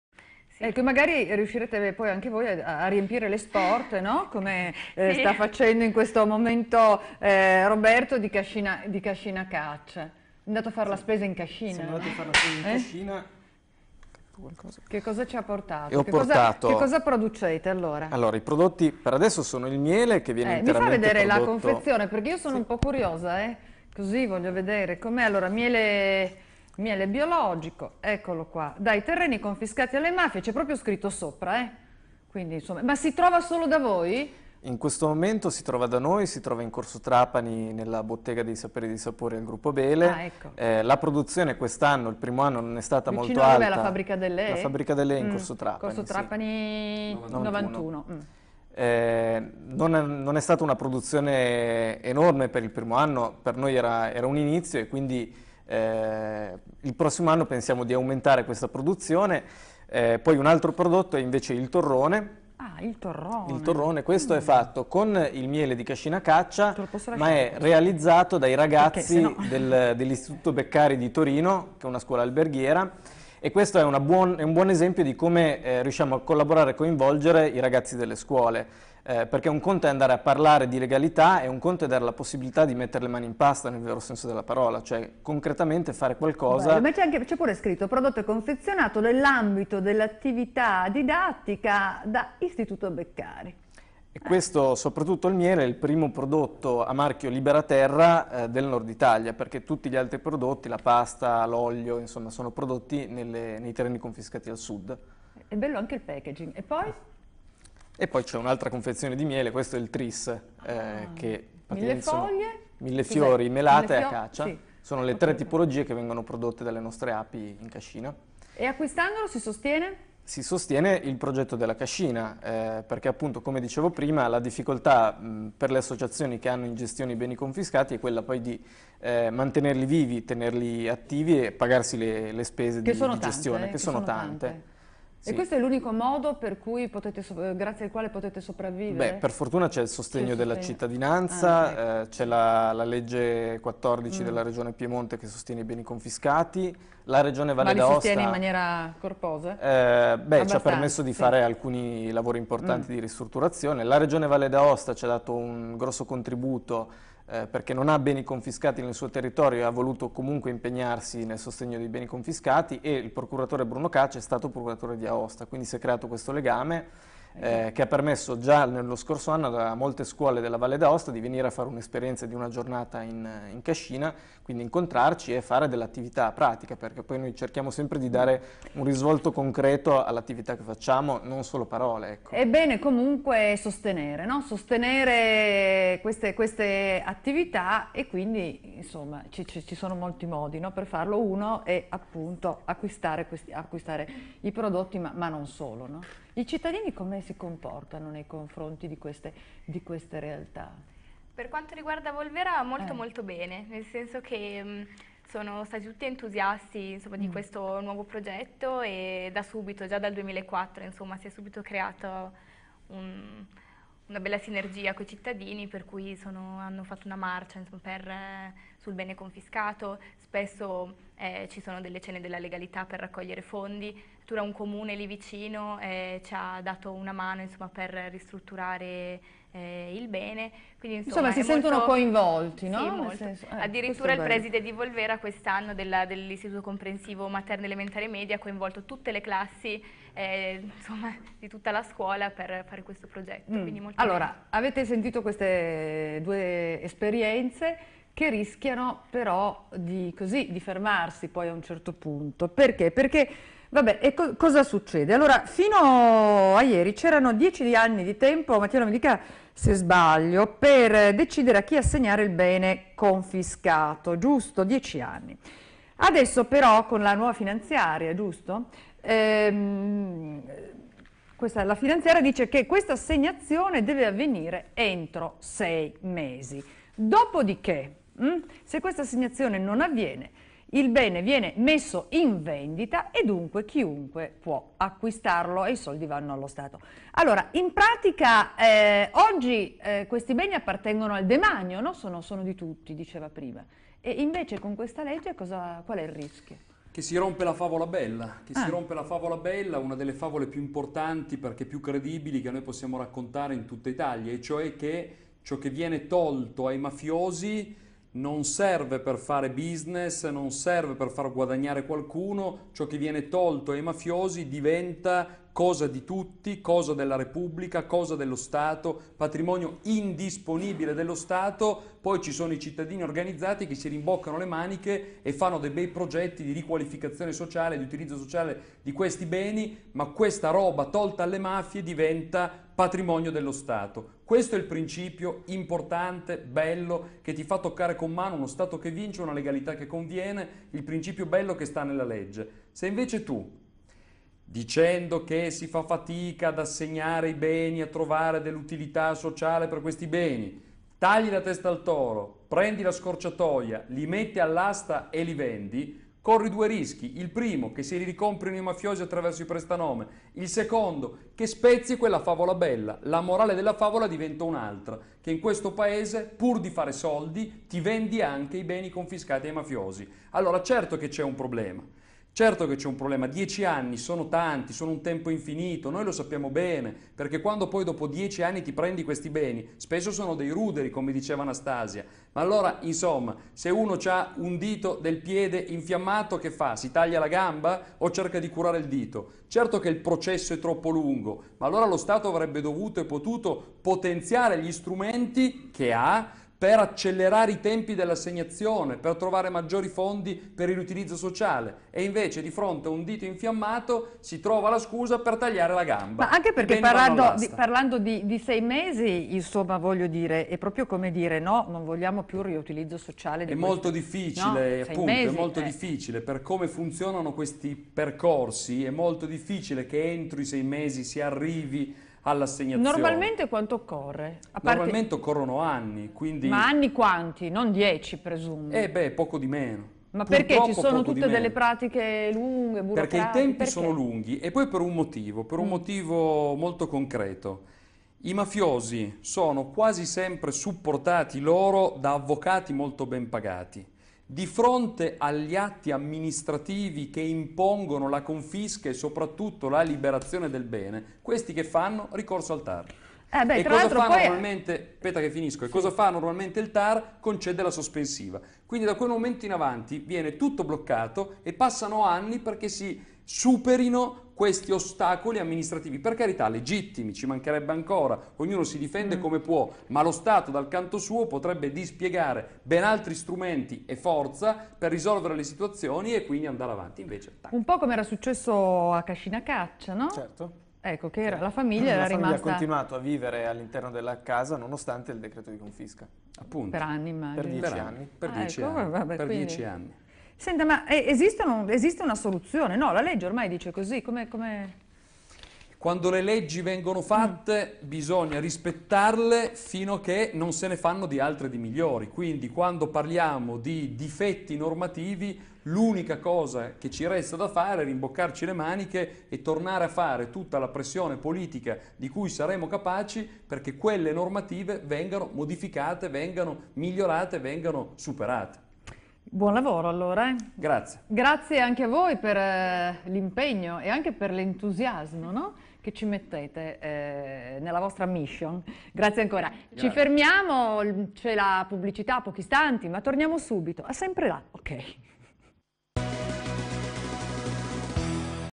I beni confiscati-Telesubalpina3
Davide, Roberto e Anastasia a Telesubalpina per parlare di beni confiscati e mafia al nord. 5 maggio 2010.
terza parte.